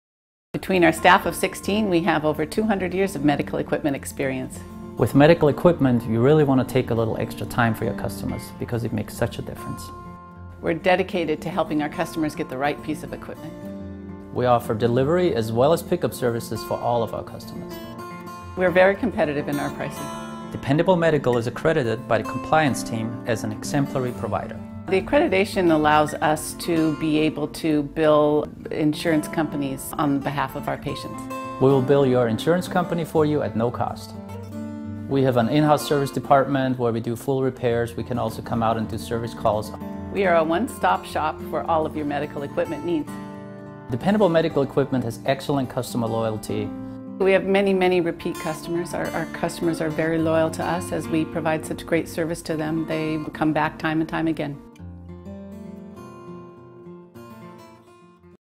Dependable Medical Equipment | Mountlake Terrace, WA | Wheelchairs | Lift Chairs
Stay healthy and in budget with Dependable Medical Equipment. Based in Mountlake Terrace, Washington, they offer the Puget Sound area a wide selection of mobility devices and medical equipment such as wheelchairs, walkers, lift chairs, braces, scooters, and canes plus expert repairs -- all at incredibly affordable prices. Free pick-up and delivery. Hate filling out forms? This accredited shop will complete all insurance paperwork and bill your insurance company directly.